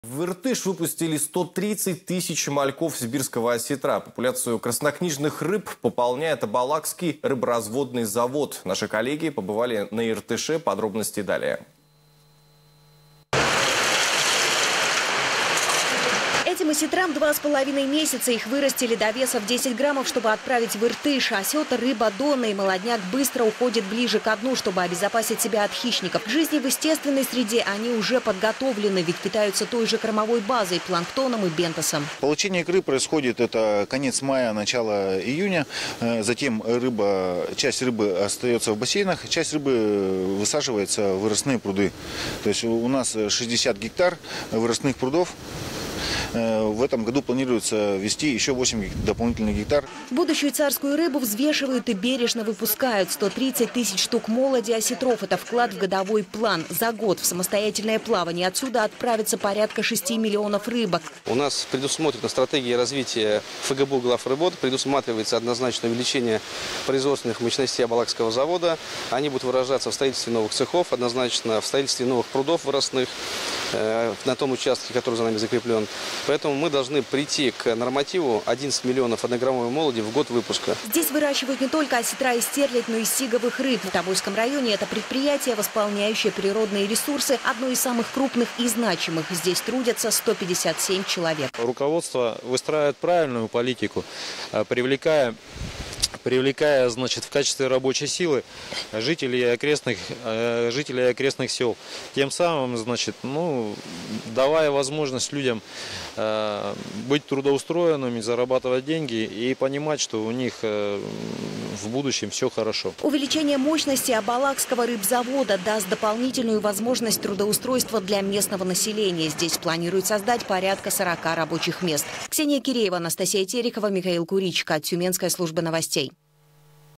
В Иртыш выпустили 130 тысяч мальков сибирского осетра
130 тысяч мальков сибирского осетра выпустили в Иртыш. Пополняет популяцию краснокнижных рыб Абалакский рыборазводный завод. Осетрам два с половиной месяца. Их вырастили до веса 10 граммов, чтобы отправить в реку.